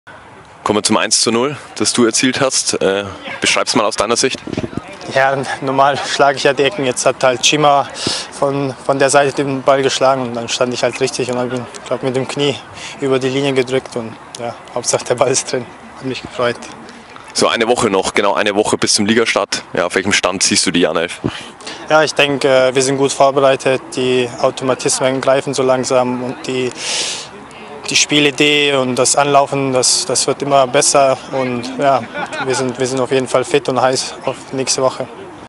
Interview mit Max Besuschkow
Max Besuschkow erzielte im Test gegen Mainz 05 den Treffer zum 1:0 und wurde vor dem 2:0 elfmeterreif gefoult. Nach dem Spiel sprach er mit Jahn TV, zum kompletten Interview